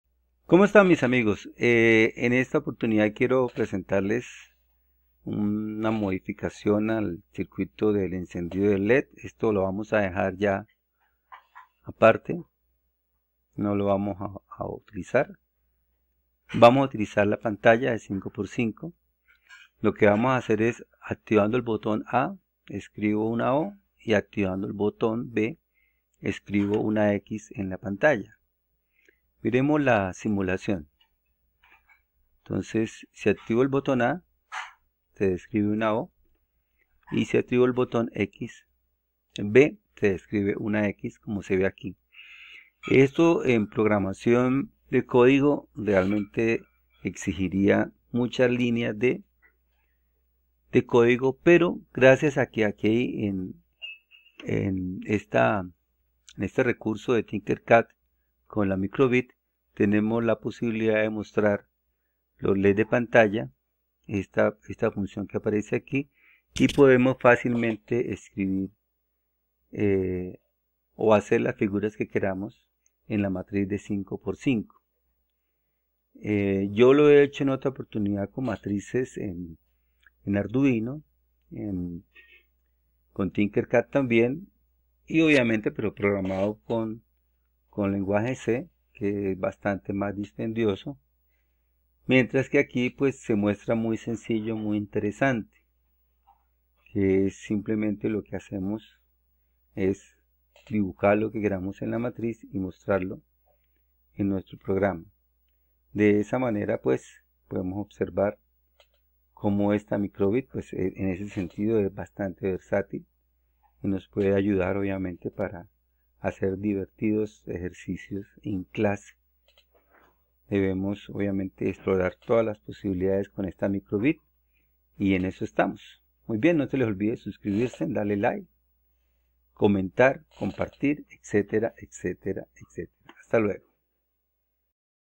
haciendo figuras en la microbit
en este video se ve como se hacen figuras en la matrix de led´s de la microbit